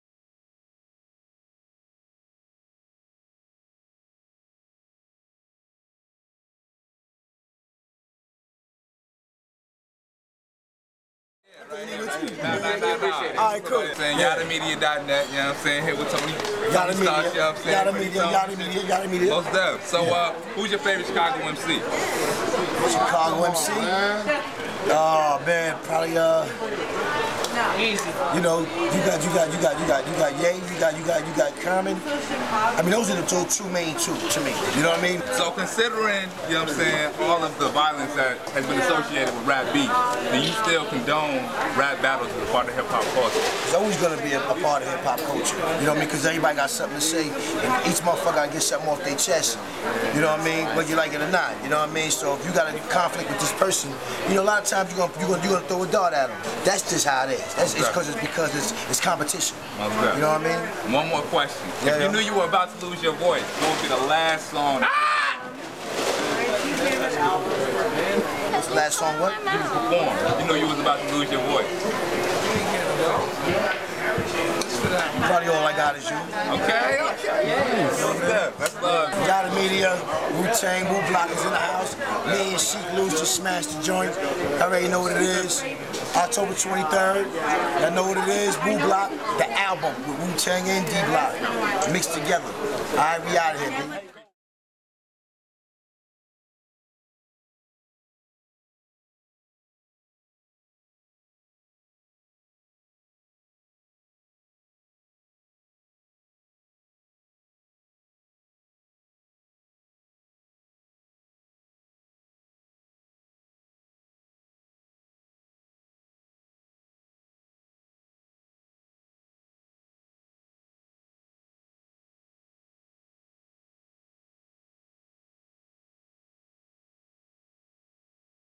Exclusive: Ghostface Killah Interview
On September 7th, 2012, our crew met up with Ghostface Killah and interviewed him after the show on the "Wu-Block Tour" in Chicago

Directed & Shot by Carl Reed

Interviewer: K.O. Johnson

Hire us for your next show!